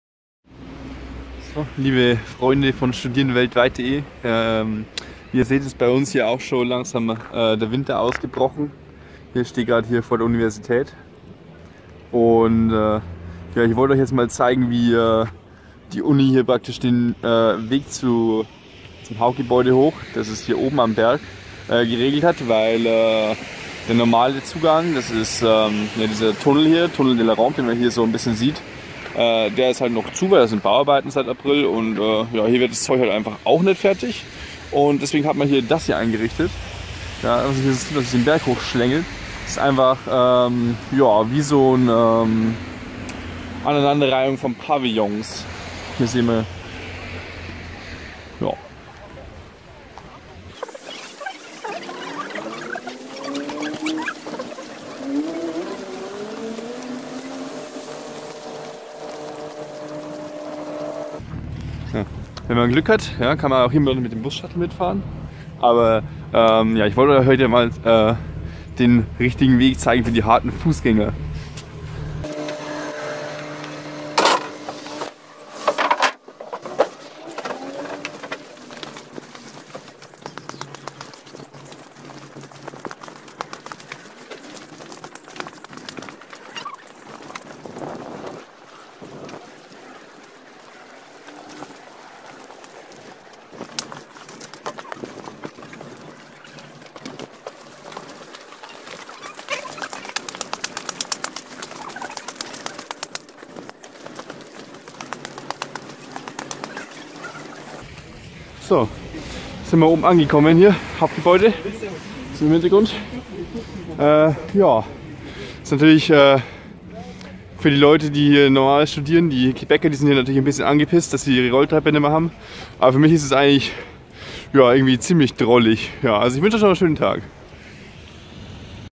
Improvisieren auf Kanadisch: Wintervorbereitungen an der UdeM
Durch die Hanglage gestaltet sich der Zugang zur Université de Montréal recht abenteuerlich, zumal der Tunnel mit der Rolltreppe renoviert wird.

Ich habe für euch einmal die provisorische Lösung auf Video festgehalten.